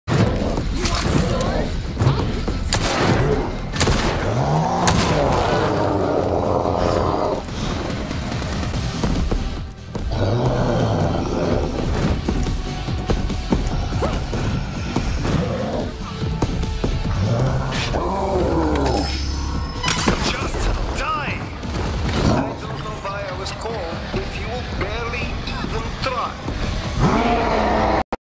He Gave Me STARS.
Nemesis showed up right as we were gathering at the exit and just started throwing haymakers.  😔